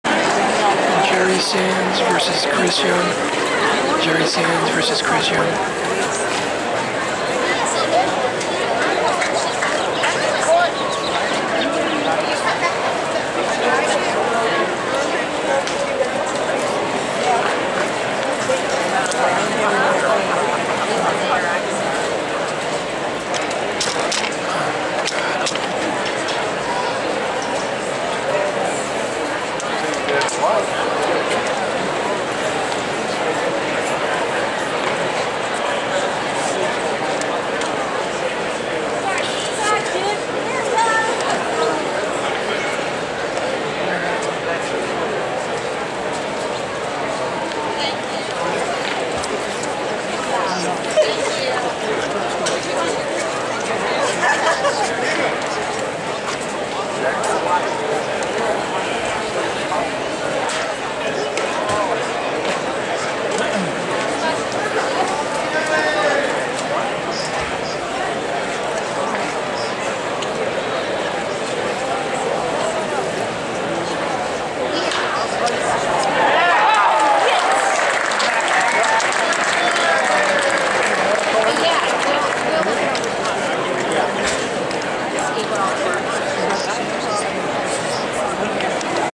Prospect Jerry Sands at bat 2015
Cleveland Indians Jerry Sands at bat VS Chris Young, Kansas City Royals. Surprise Stadium, March 2015.
Sands was drafted by the Los Angeles Dodgers but was later traded.
Sorry for the "fan interference" the usher and the couple taking a picture had no clue I was filming and she kept moving to block my shot.